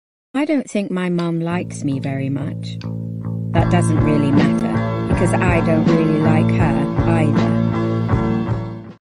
I Don’t Think My Parents Like Me Very much // ST // Mike Angst
Audio from the reading of Solitaire (I think)

Anyways, family issues with Michael time!